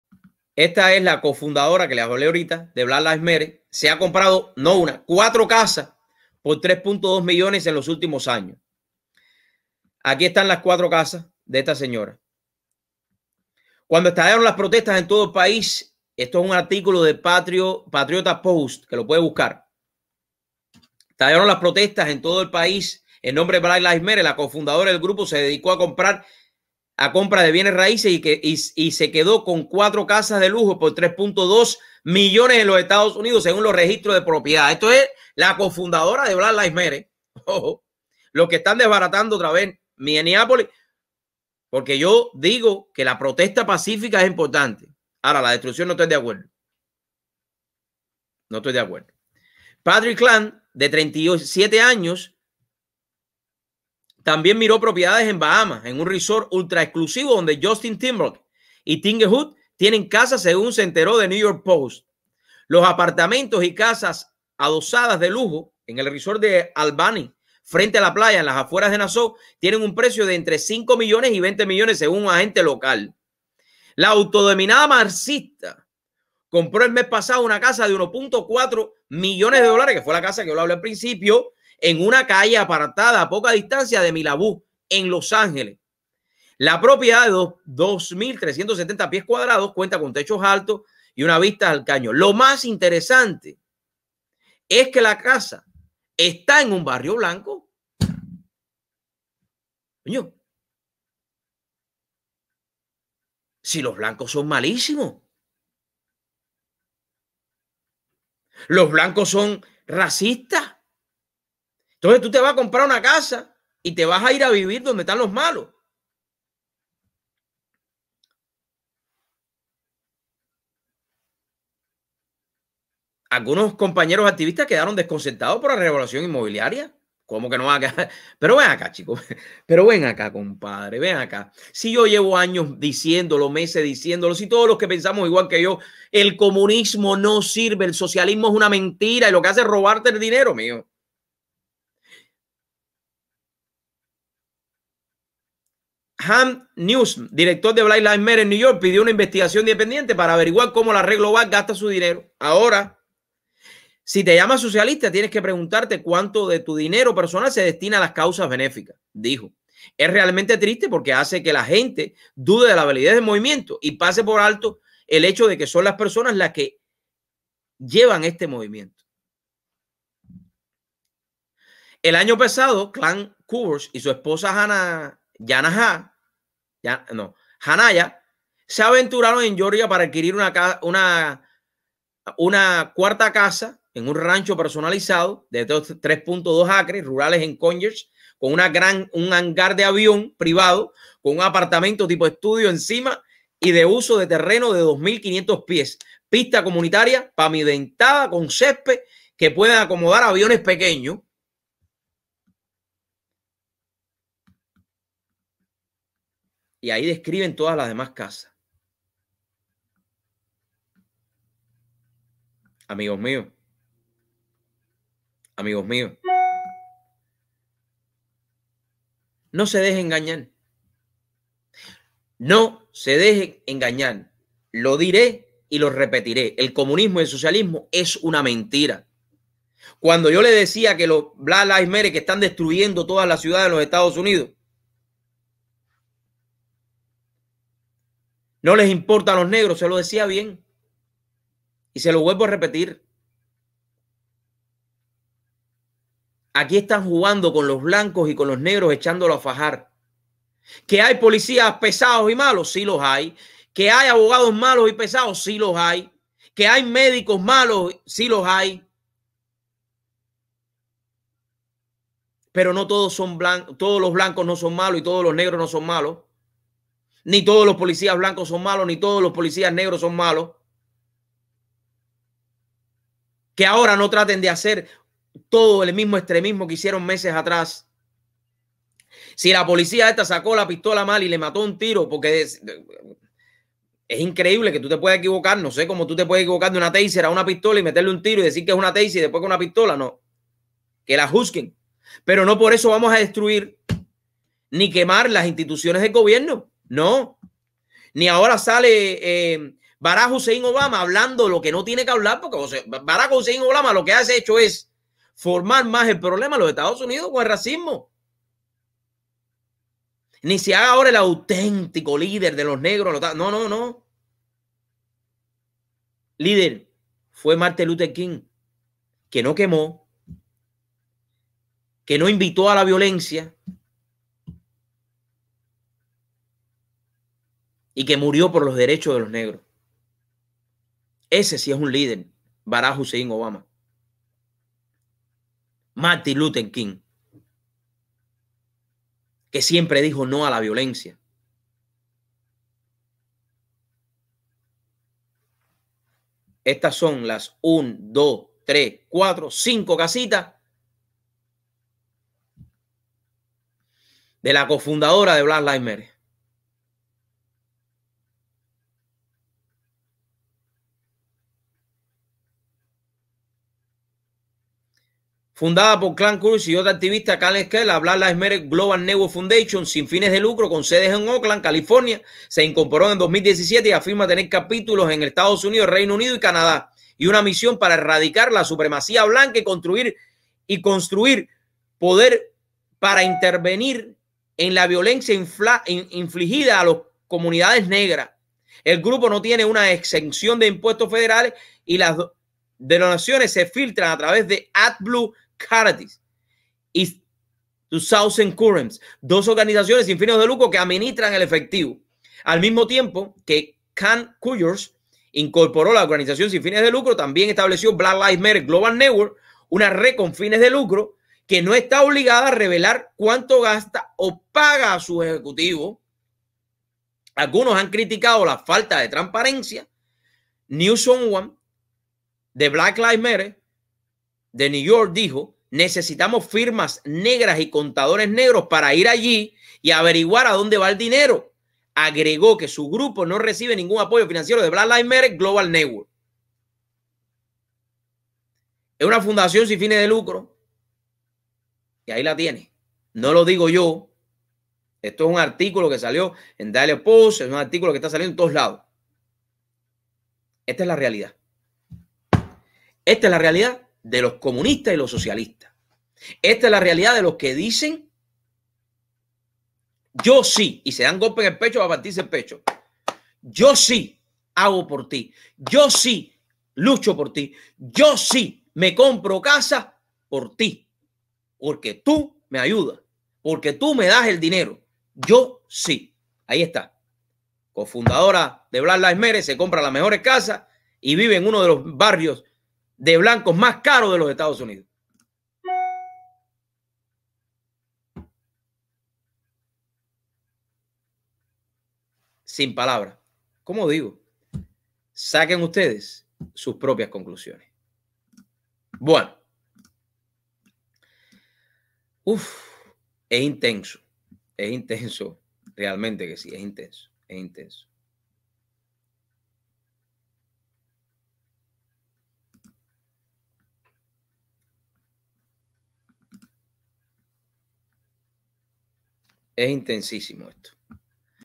Cofundadora de BLM es comunista pero se compra 5 propiedades de casi 6 millones.
💎Sign up for our newsletter to receive accurate news without spin. Esta es la manera mas segura para estar en contacto